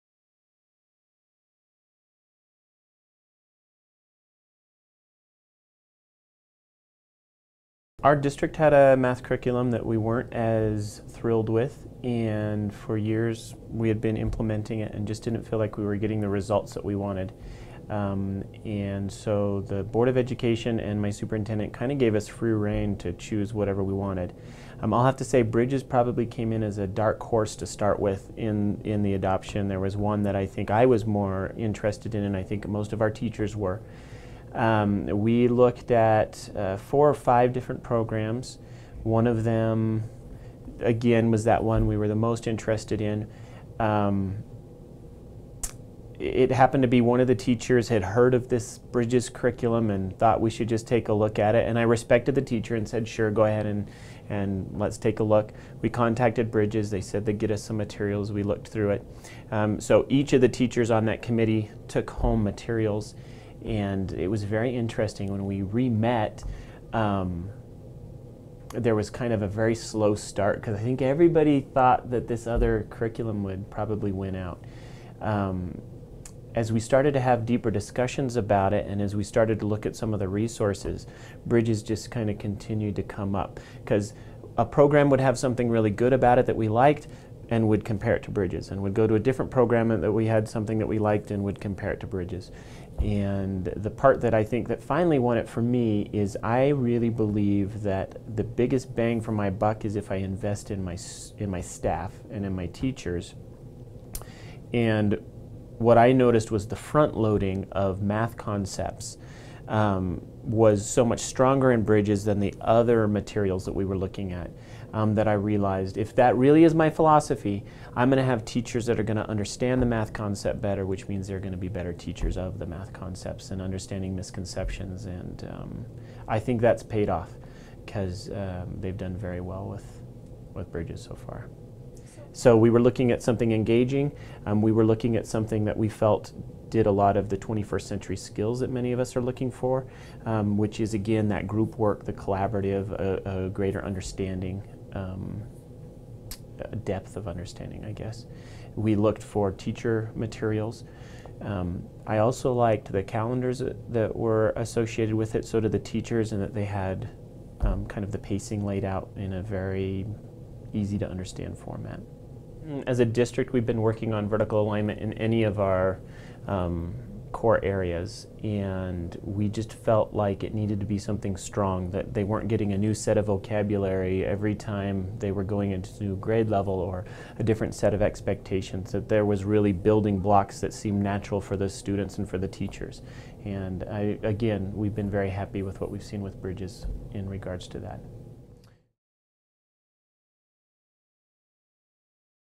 Bridges in Mathematics Testimonials: How Did You Choose Bridges
Principal Dan Seegmiller describes how his school chose Bridges.